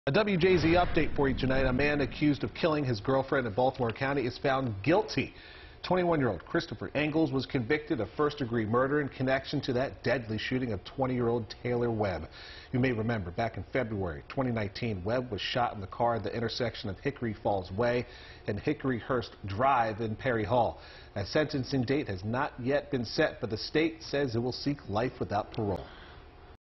21-Year-Old Christopher James Engles Convicted Of First-Degree Murder
Officials announced that 21-year-old Christopher James Engles was convicted by a jury of first-degree murder in the 2019 shooting death of Taylor India Webb.